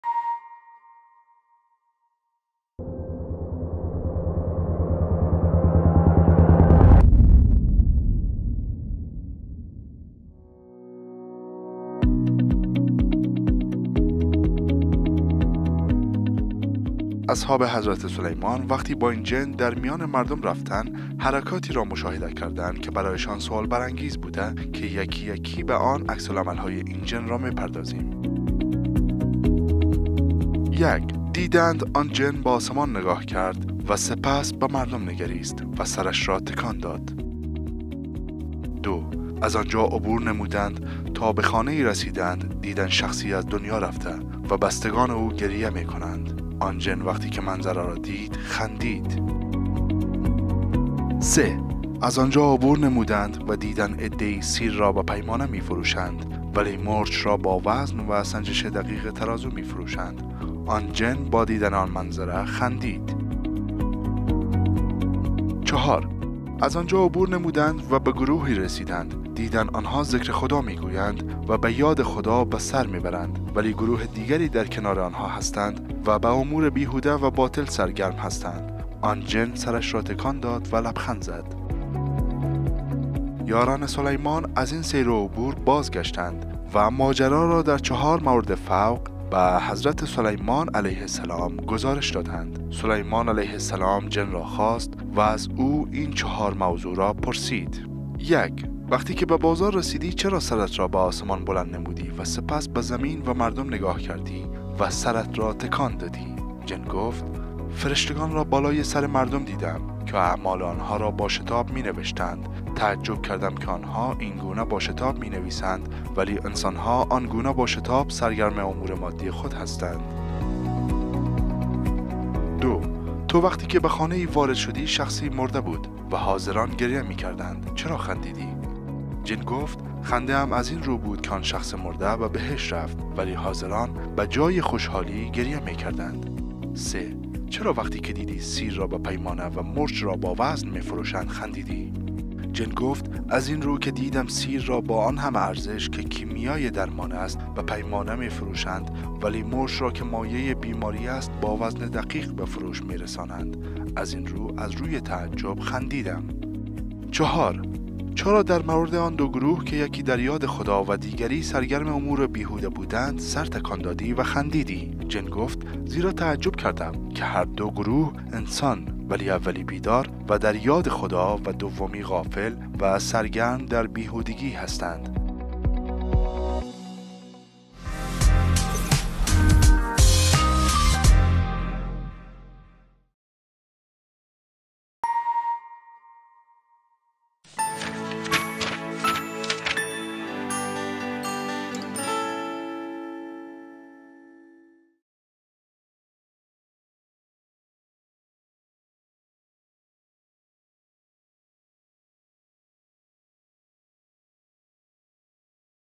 NEXT MEDIA _ داستان یک جن با حضرت سلیمان
حضرت سلیمان علیه السّلام از پیامبرانی بود که خداوند او را بر جنّ و انس مسلّط نموده بود.
معلومات عمومی
بهترین ها
نکست مدیا
Next MEDIA